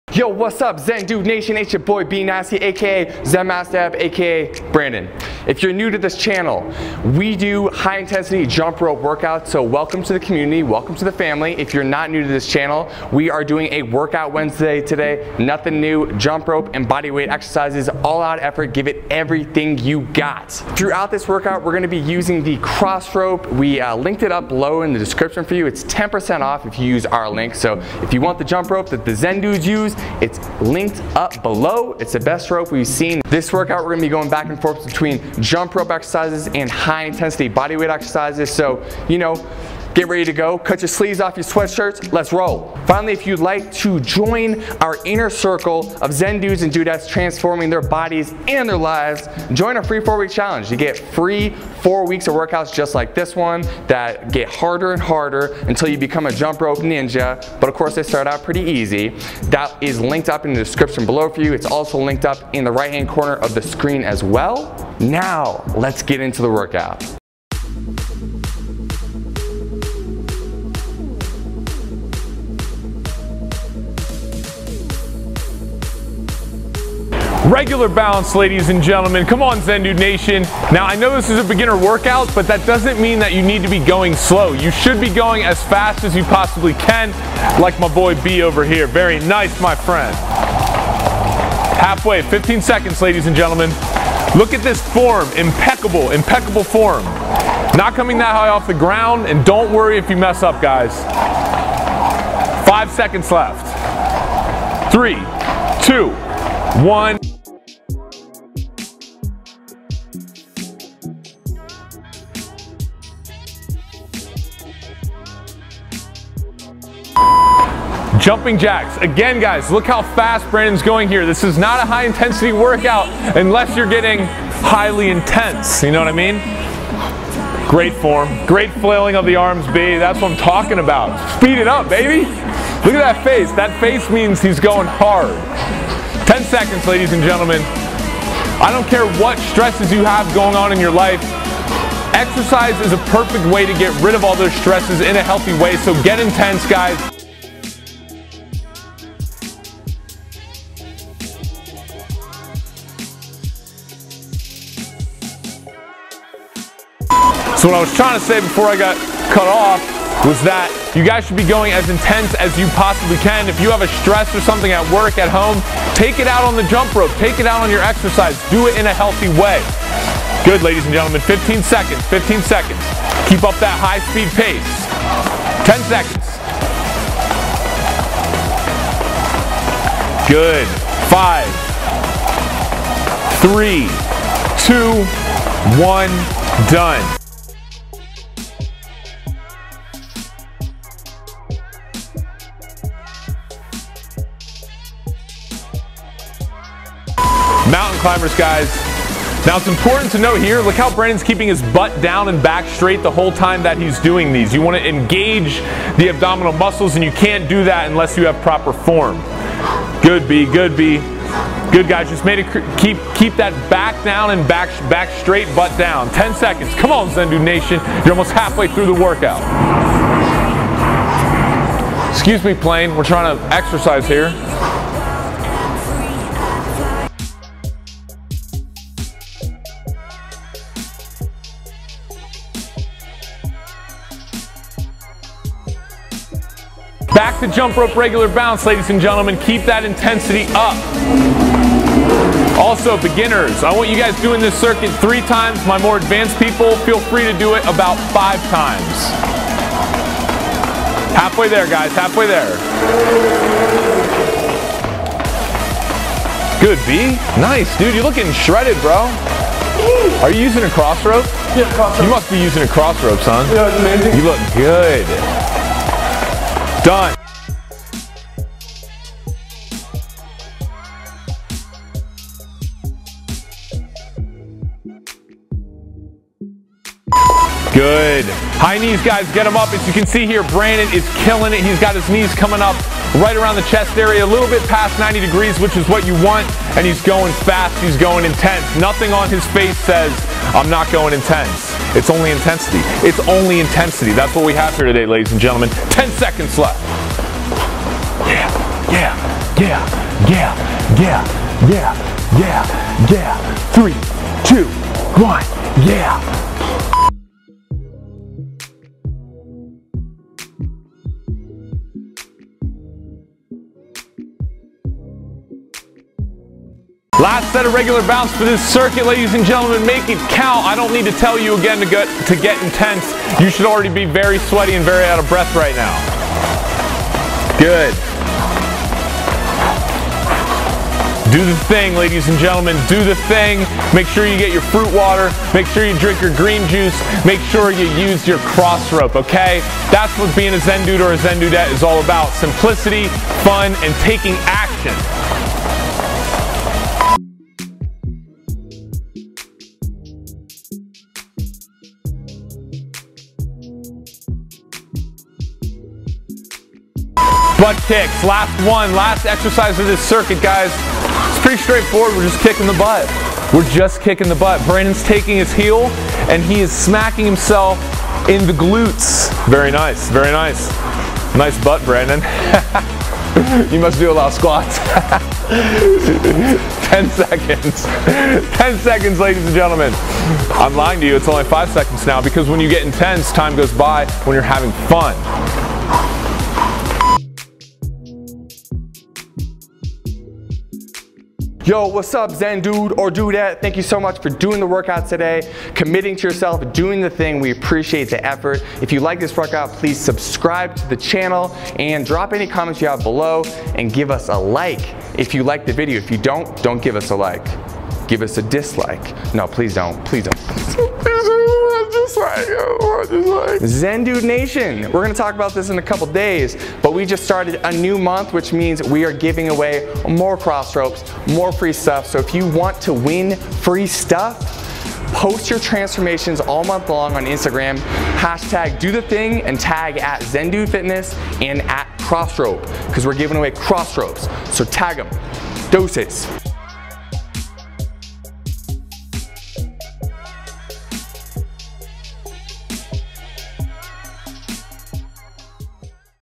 HIIT Workout For Beginners
This workout is specifically for beginners who want to burn a lot of calories in a short amount of time, but don't want to have to do any crazy complex exercises. All you need is a jump rope, a flat surface and some open space!

30 seconds - jump rope - regular bounce
30 seconds - jumping jacks
30 seconds - jump rope - regular bounce
30 seconds - mountain climbers
30 seconds - jump rope - regular bounce
30 seconds - high knees
30 seconds - jump rope - regular bounce
30 seconds butt kicks

**rest 10 seconds between exercises
**complete circuit 3-5x (3 beginner, 5 for advanced)